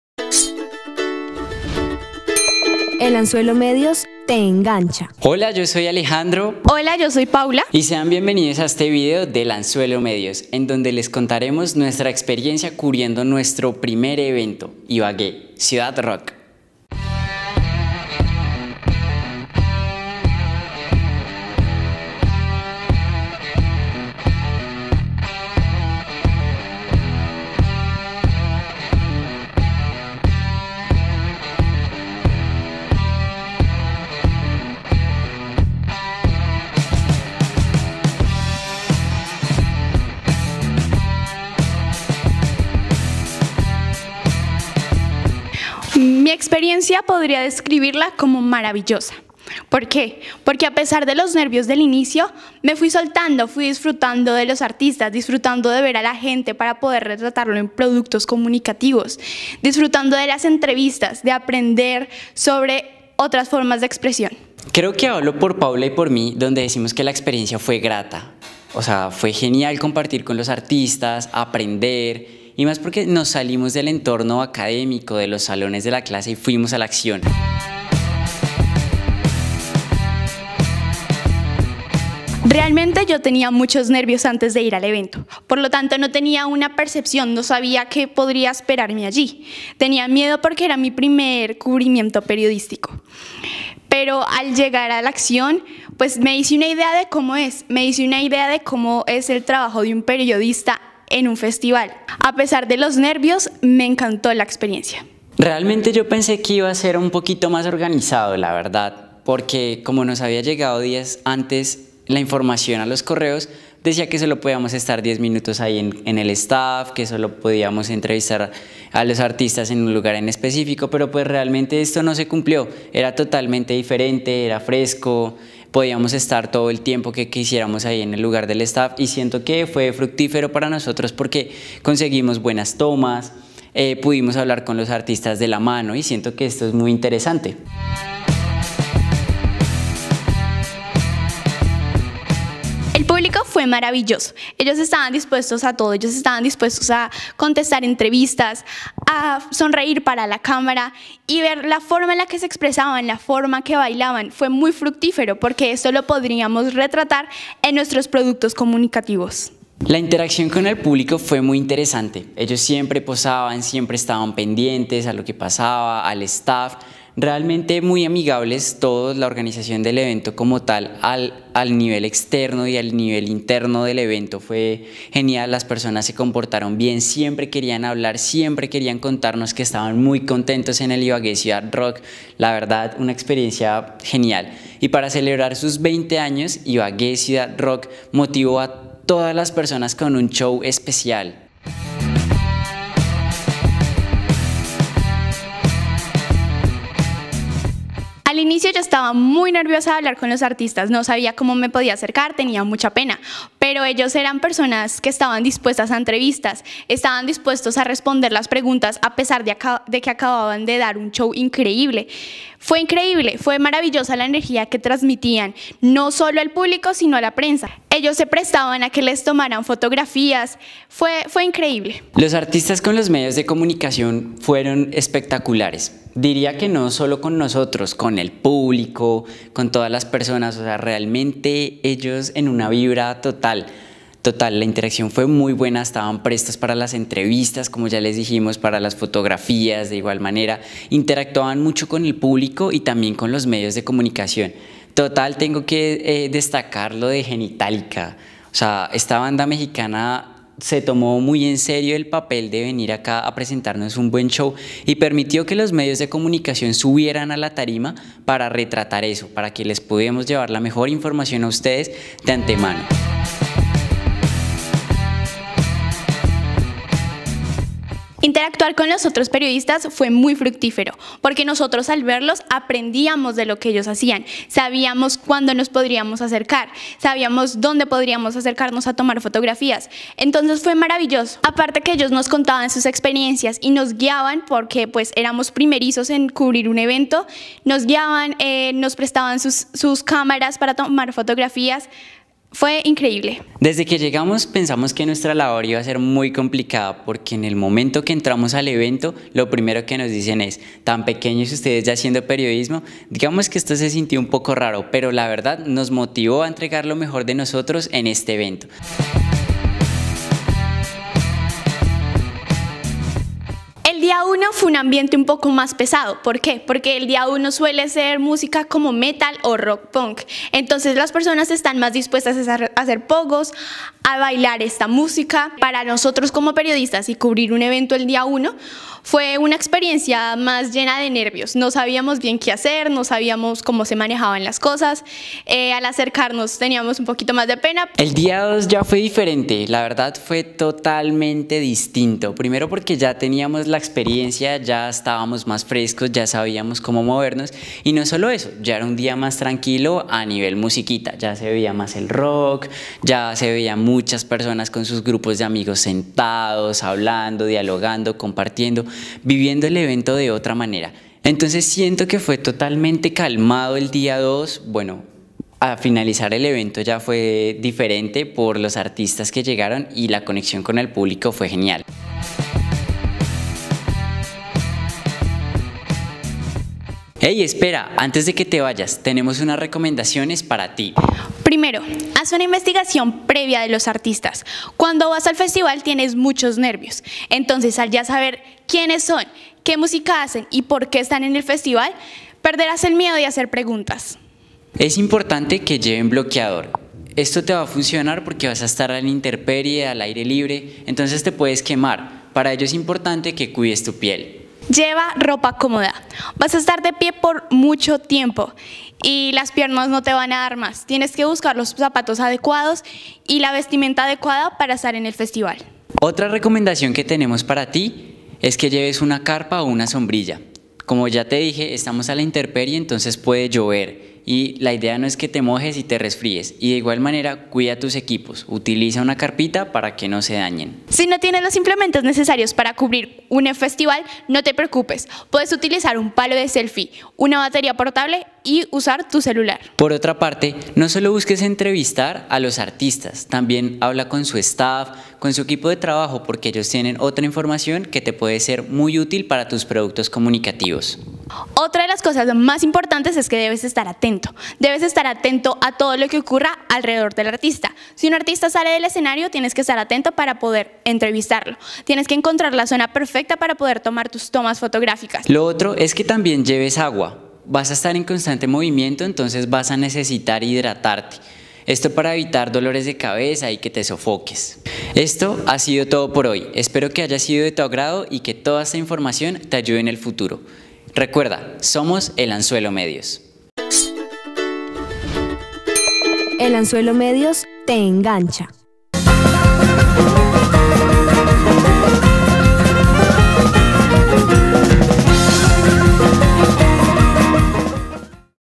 Nuestra experiencia en el ICRXX
Paula Núñez y Alejandro Vargas se aventuraron a cubrir su primer festival de rock y nos comparten sus sentimientos y vivencias.